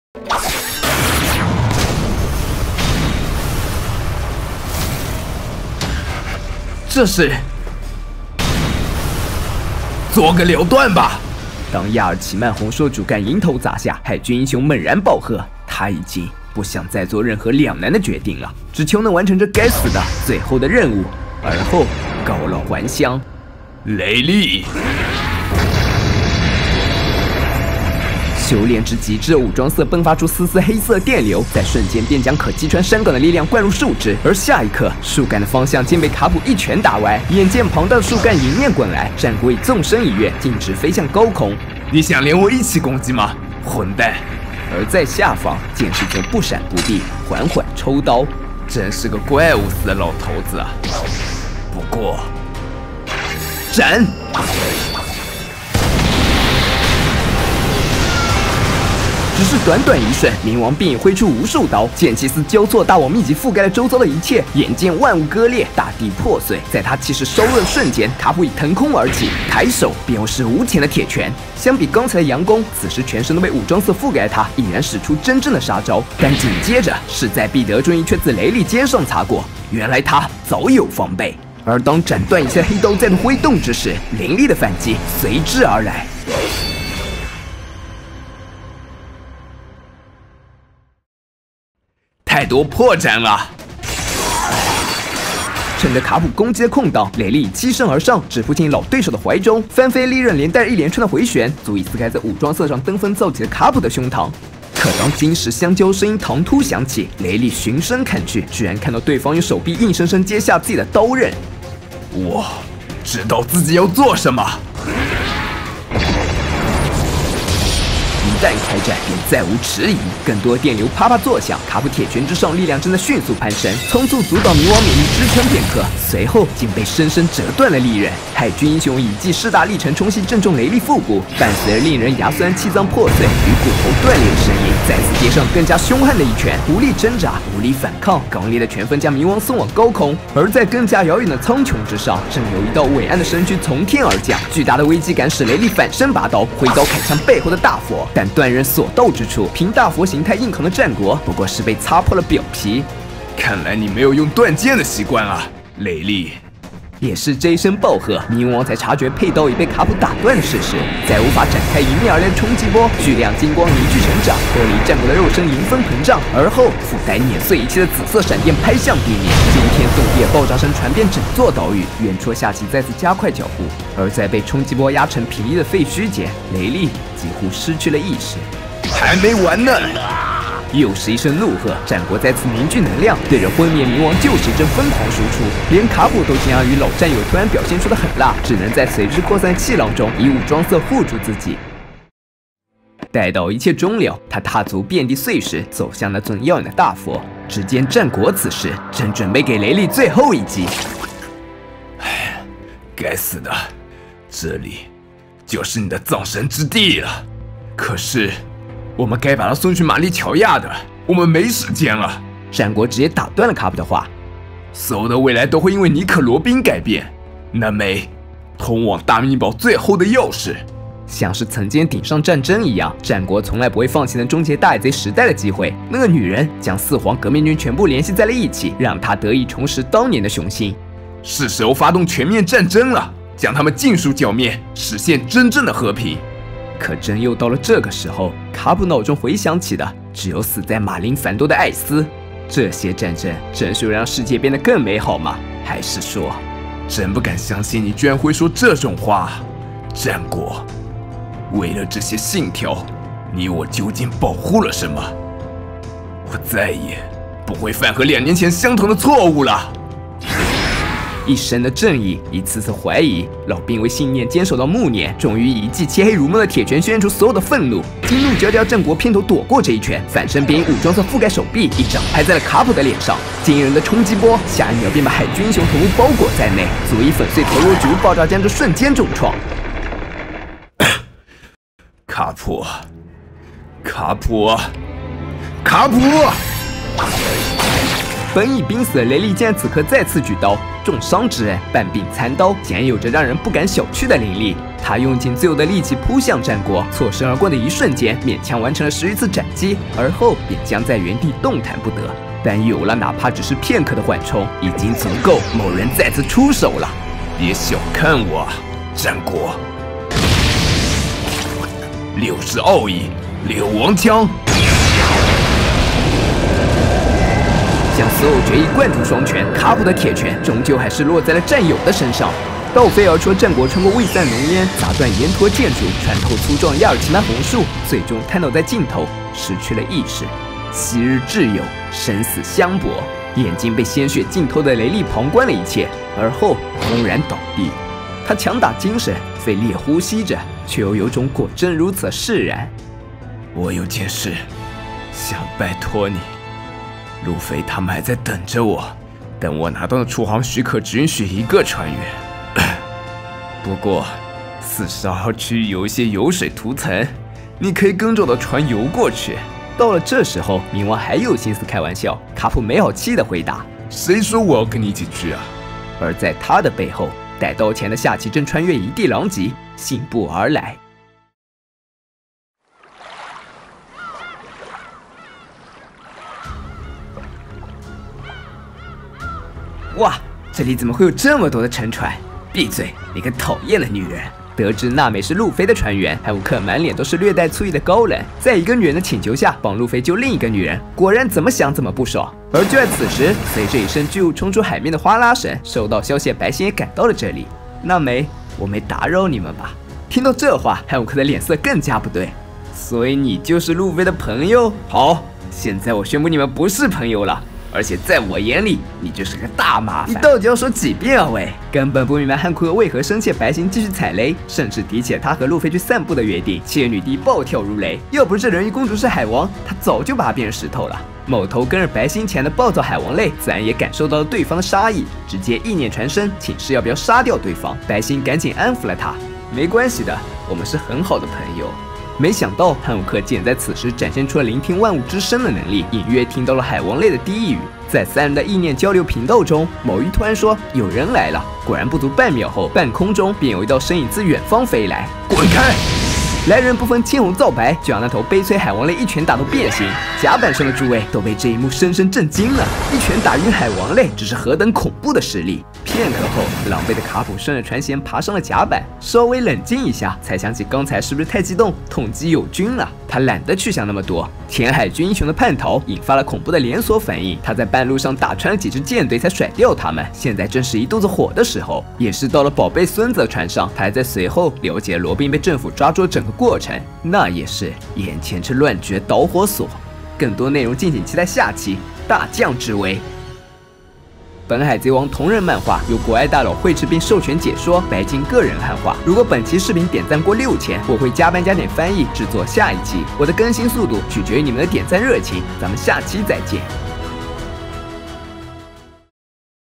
卡普叛逃！冥王雷利瀕死前的驚天一斬，佛之戰國昏迷落敗！【海賊王漢化同人】
這一次，卡普選擇了自己心中的正義
本期視頻是海賊王的同人漫畫解說
由超優秀漫畫師@mas_ricky_acong_subroto绘制并给与授权